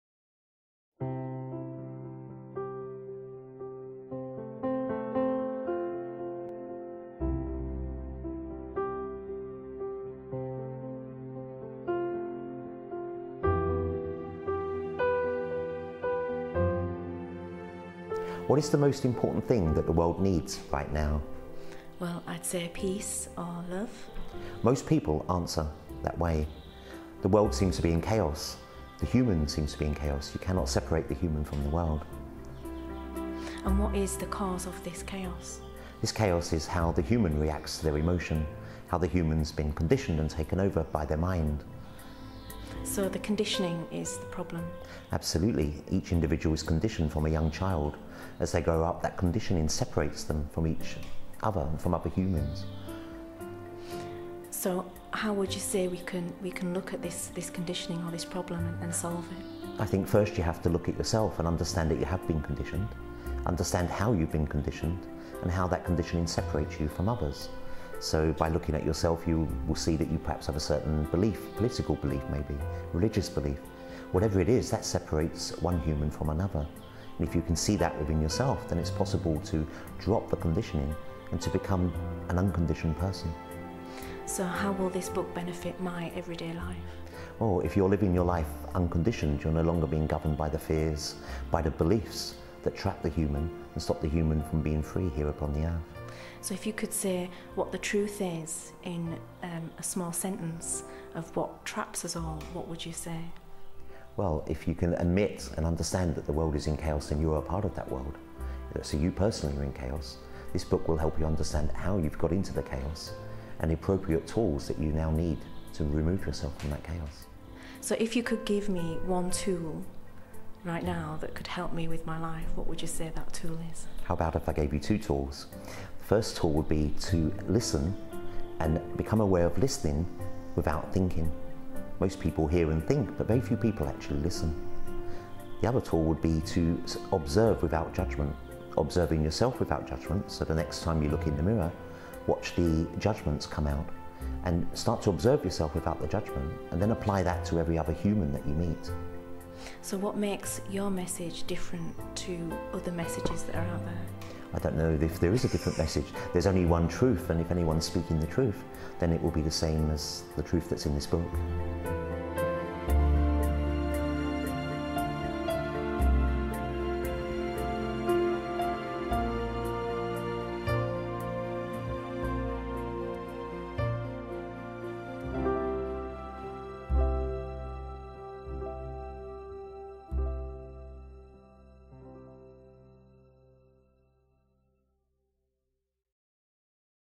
A short dialogue with Mike Robinson on his new book: The True Dynamics of Life
A look at the chaos of humanity and how individual beliefs and conditioning are what keeps the chaos alive on this planet.